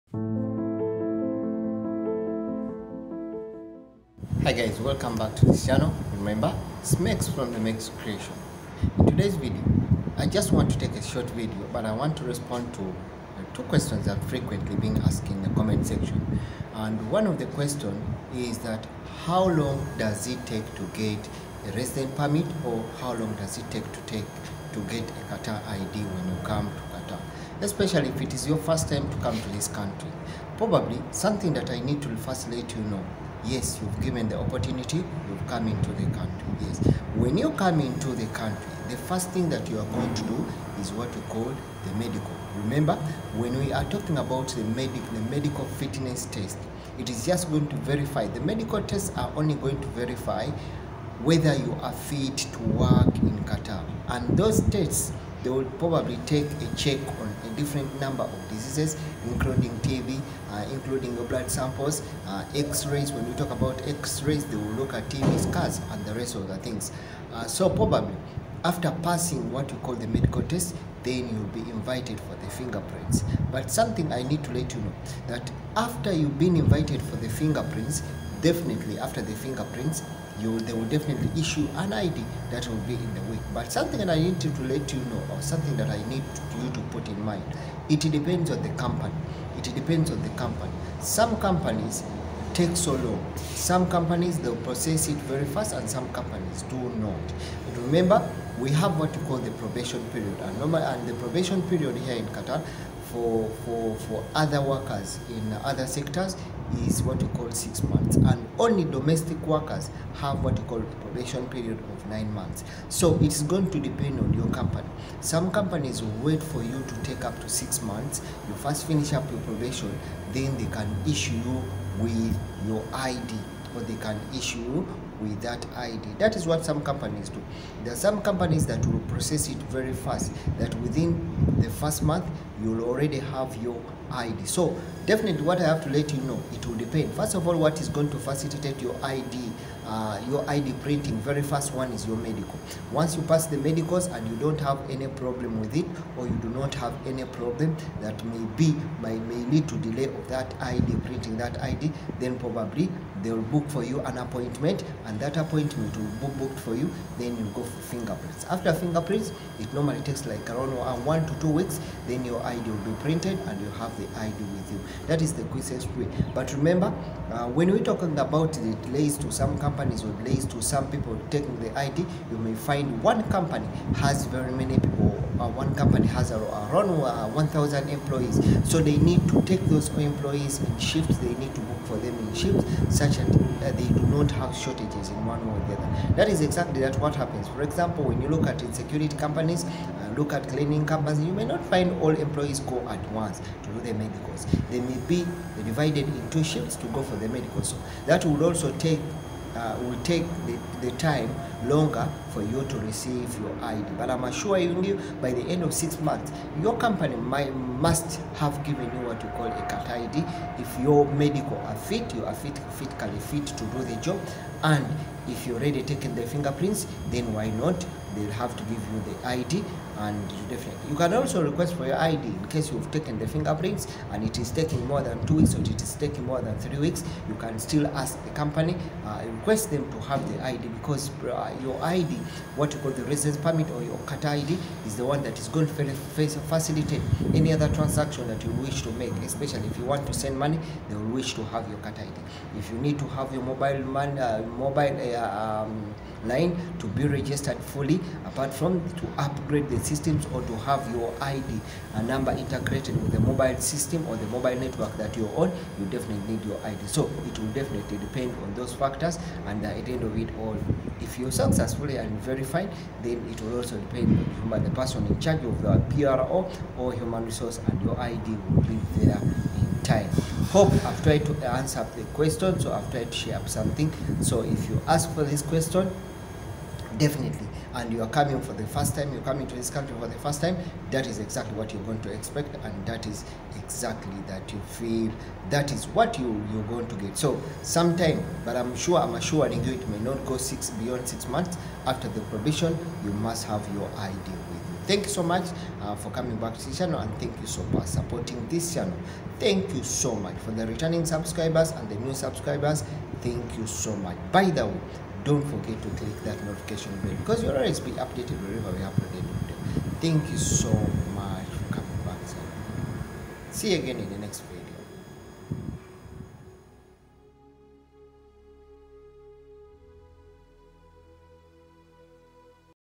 Qatar 2022; How long does it take to get the resident permit ( Qatar I'd) | Requirements in Qatar
Everyone living in Qatar, from infants to the elderly, needs a Residence Permit. Non-residents and tourists can stay in the country for as long as their visa allows, but may not work or stay beyond that period without an RP.
The good news about the RP is that employers handle the bulk of the paperwork. Once you have found an employer that is based in Qatar and wants to hire you, they begin the paperwork to become your sponsor. Once the process is underway, you will have to travel to Qatar because the paperwork must be completed with you in the country. It is very important that you don’t leave the country until the process isYou will enter Qatar on a single-entry visa and complete the required steps once you arrive. You will also need to take a blood test to check for communicable diseases, have a chest x-ray, and provide fingerprints. It is important to know that if you are found to be positive for HIV, you will be denied an RP and deported.
Although your employer handles the bulk of the process, be sure to confirm with them all the steps that you should be handling on your end. The process of getting a Residence Permit usually takes between two to four weeks, however, it can take longer. Once you (or the employed person) get the RP, then you (or they) can sponsor your family to live in Qatar.
The main requirement for getting a Residence Permit is an employment contract from a Qatari employer. Besides that, you will need your original passport (and copies), four passport-sized photos, and whatever your employer asks you to provide (attested educational documents, marriage certificate, and so on). Your best bet is to follow your employer’s lead. Residence Permits cost QR 1,000 per year.
When you first arrive in Qatar, you will enter the country on a temporary visa that your employer will have organized. Once you are in the country you will complete the steps to convert that into a Residence Permit. Keep a steady communication with your employer to make sure they have everything they need, as this will avoid delays and keep you in the loop.
lam always grateful you watching my videos.ns